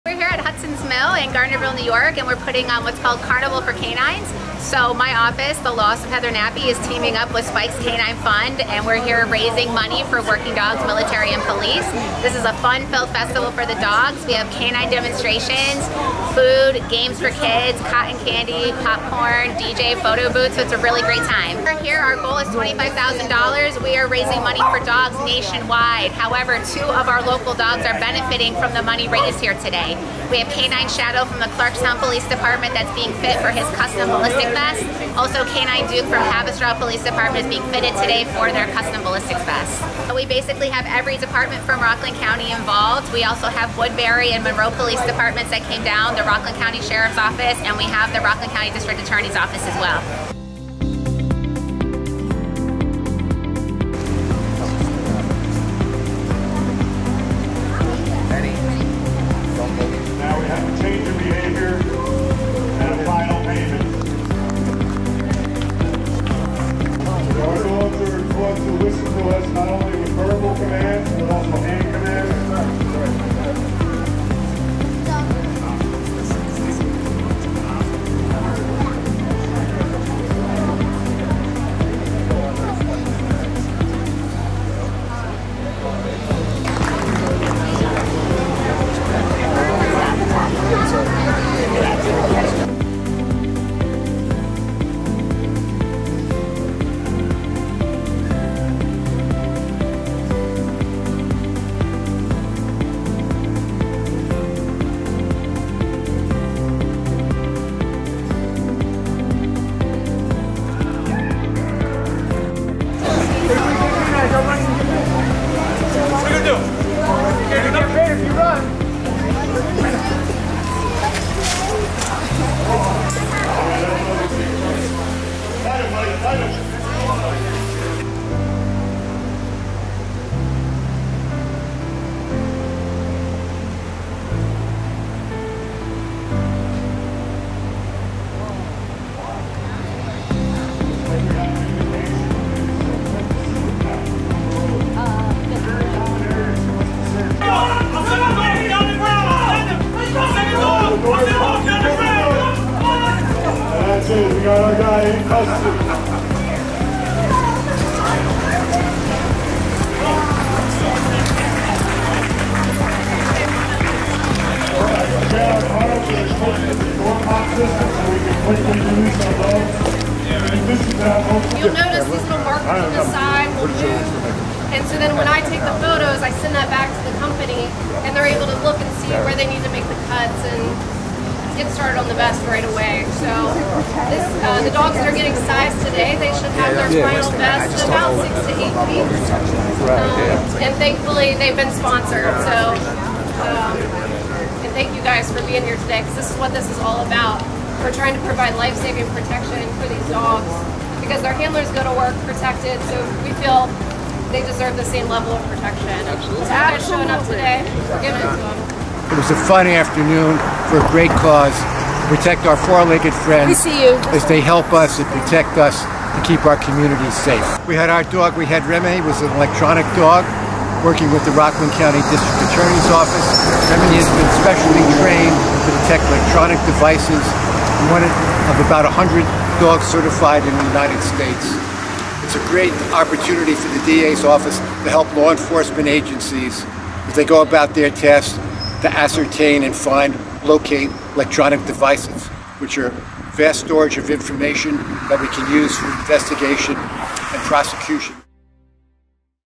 Carnival for Canines to Benefit Rockland County Police K9s and the Spike's K9 Fund
District Attorney Tom Walsh and Chief Assistant District Attorney Patrick Frawley had the pleasure of attending the Carnival for Canines benefit to raise money for Rockland County K9s and the Spikes K9 at Hudson's Mill in Garnerville. The cost for upkeep of K-9's can be very expensive, but they provide an invaluable and non-lethal tool for law enforcement. Clarkstown Police Department K9 Shadow and Haverstraw Police Department K9 Duke were fitted and will be receiving bullet-proof vests from Spikes K9 due to the money raised from the benefit.

Rockland County District Attorney's Office K9 Remy and handler Detective Tim Hayes participated in the event and showed her electronics detection skills and got to interact with attendees.

A great job by event organizer The Law Office of Heather M. Nappi, PC, all the vendors, and our incredible dog handlers and their k9s who serve and protect our community.